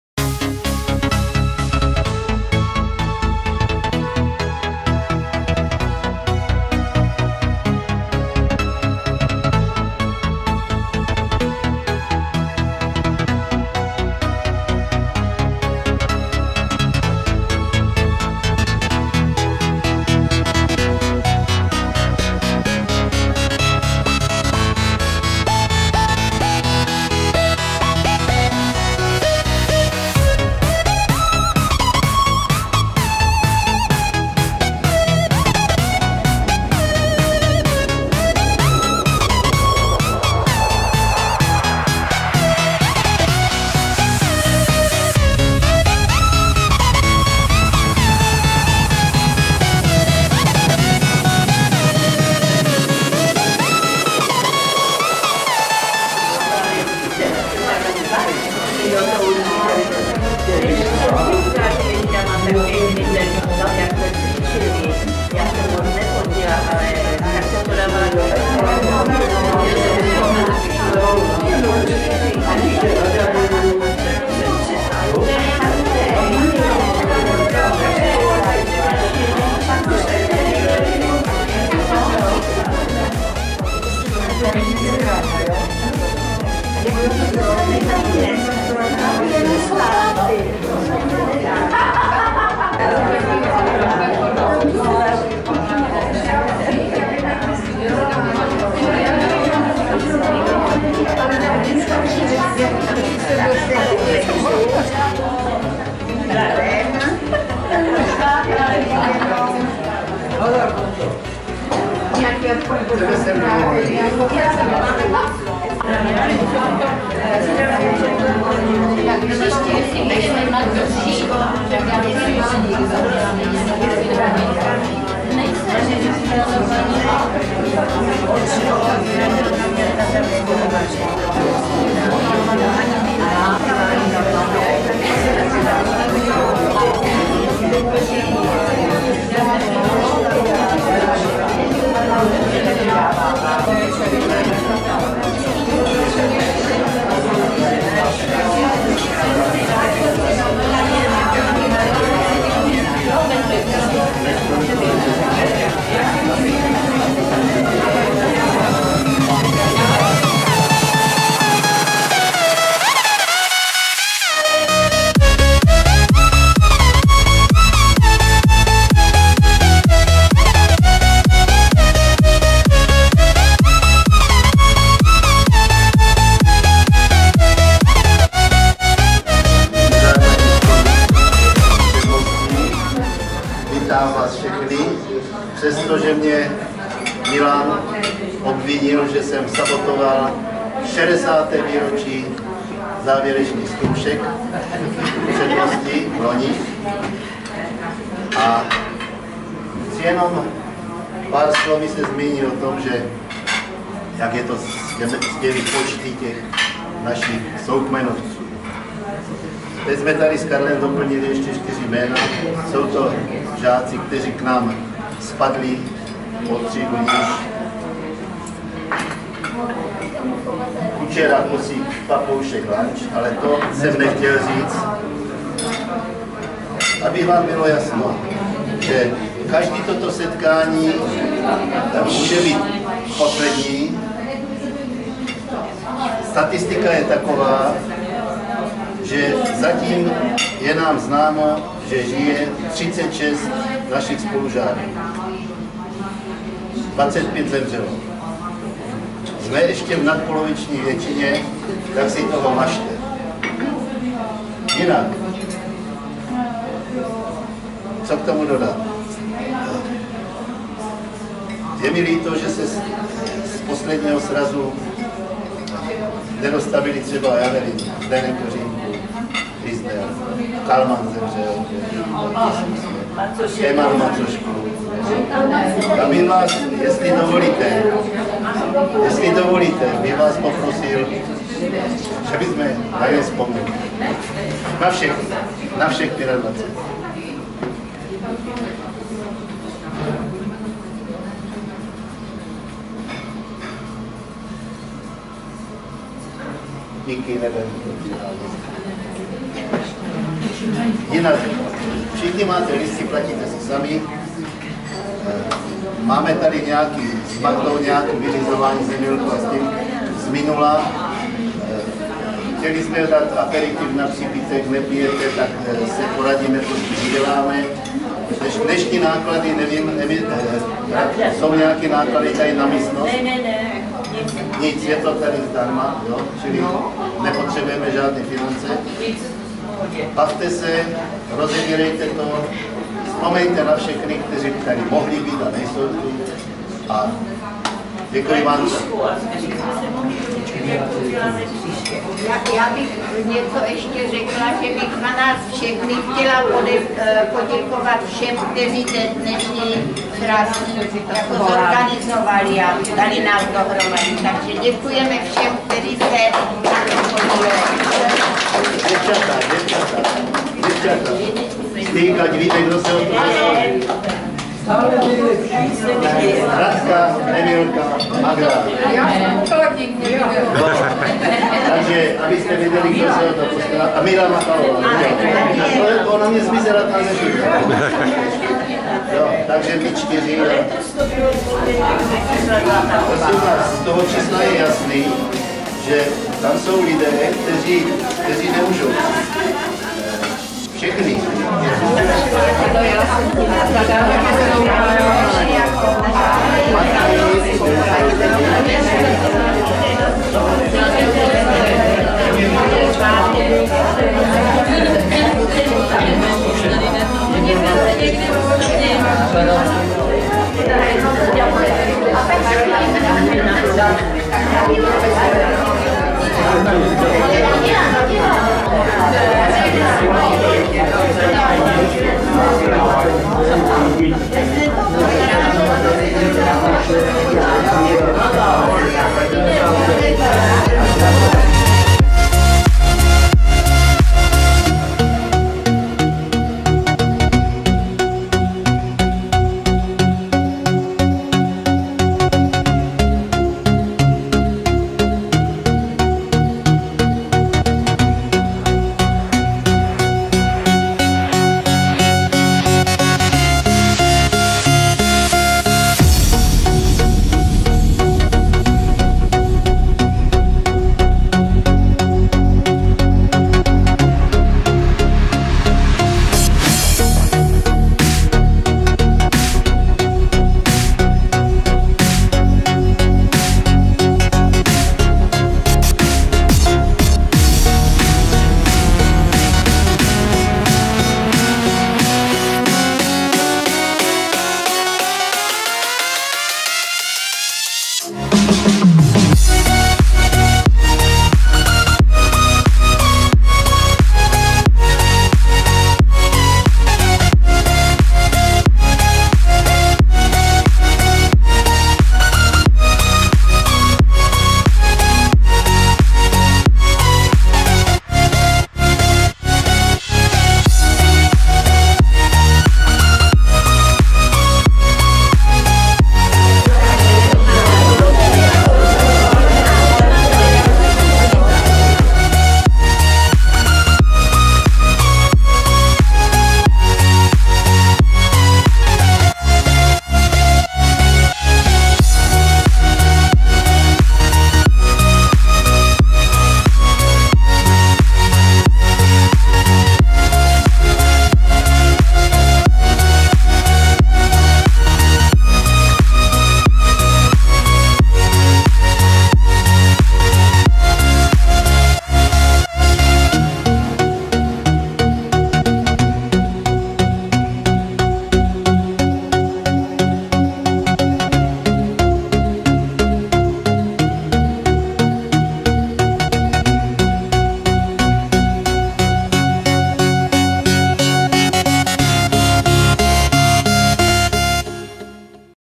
sraz 75 ti letých OŠ Předmostí
sraz spolužáků z osmiletky Předmostí 1947 až 1957